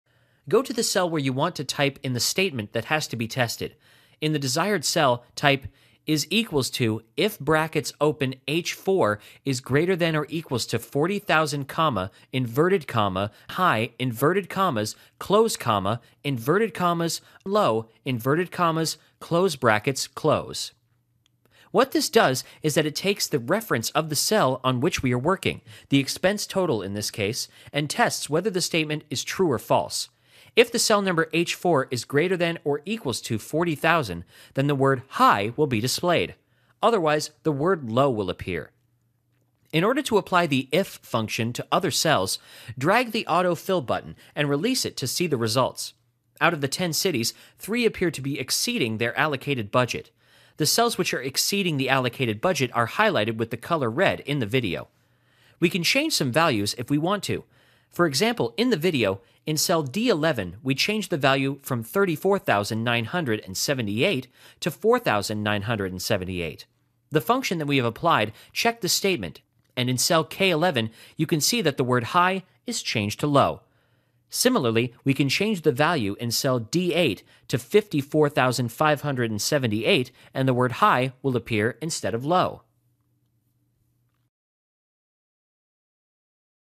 How to Use IF Function in Excel 2010
Learn how to apply the "IF" function to your Excel worksheet. Work with a certain condition and apply logical reasoning.

In Excel, there are more than 300 functions. There is one called the "if" function. It allows you to apply logical reasoning into the data and by that you can perform various tasks. It allows you to test an argument; the argument can either be true or false. We can apply a condition which will be tested. If it is true or false, different results will be shown accordingly. Such kind of logical tests can be applied by using the "if" function.

Follow this step by step tutorial to learn how to use if function in excel.

Step # 1 -- Use the IF function

In this worksheet we have values for the expenses that were incurred; we want to write "high" if the total value exceeds $40,000, otherwise we want to display "low". Thus we need to insert a condition. In a new column, write "Evaluation" and under that write "=if(h4 more than 40000,"high","low")". This logical test argues that if the value in cell "h4", which is the total of all the expenses incurred during the 6 months period in New York City, is greater than "$40000", then write "High", otherwise write "Low".

Step # 2 -- Evaluate the rest of the cities

After writing the function in Excel, press the "enter" key and use the auto fill handle to apply the function for the rest of the cities. In this tutorial, only three cities are showing "high" value and the rest of them are showing "low" value in the "Evaluation" column. The cities "Los Angeles", "Washington D.C" and "Miami" have totals which exceed the limit which is $40,000.

Step # 3 -- Change the values

In this tutorial, we changed the values in cell "d11", and "d8". In cell "d11", we changed it to around 5 thousand so that the total may drop and "low" will be shown in front of it.
In cell "d8", we increased the value so that "high" will be written.

Step # 4 -- Clicking on All items

After you make the changes, you will see that "high" and "low" will also change depending on the total value. As soon as you will change the values in the cells, the logical test in the "if" function will be applied to the new total and true & false reasoning will be applied to the totals.